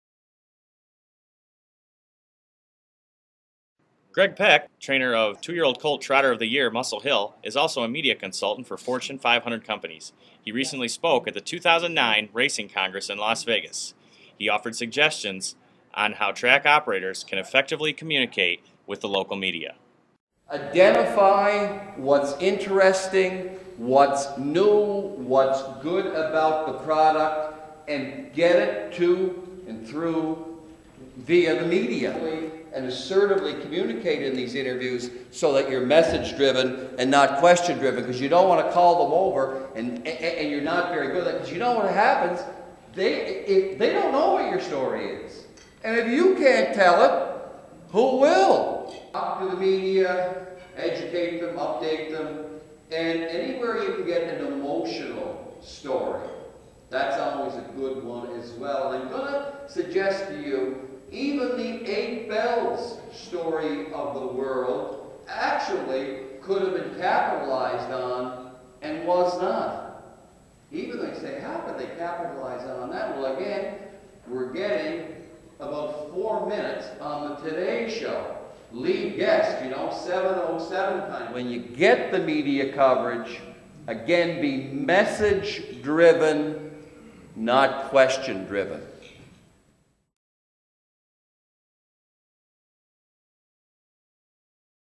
Racing in the Media -- USTA Harness Racing
Greg Peck, media consultant, speaks at the Racing Congress 2009 in Las Vegas. His topics include how to enhance racing's presence within the media.